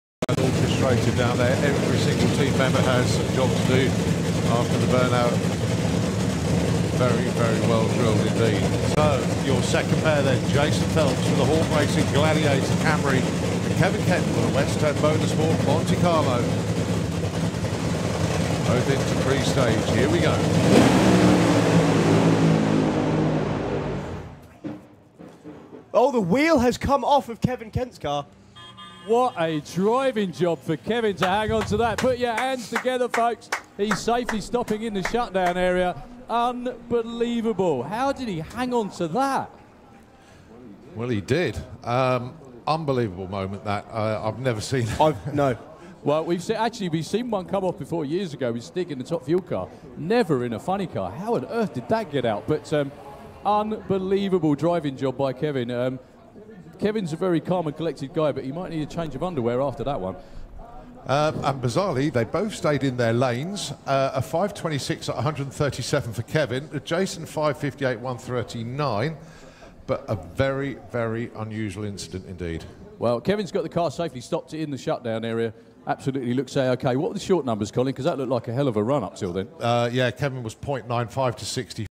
WHEEL FALLS OFF & A Smokey Ambulance! - Kevin Kent Incident | Santa Pod 2023
Halfway down the run, Kevin Kent's left rear wheel falls off and follows him down the track.  Then, an ambulance drives down to check on him and seems to be very smokey, hard to say if it locked the brakes or if the engine was smoking.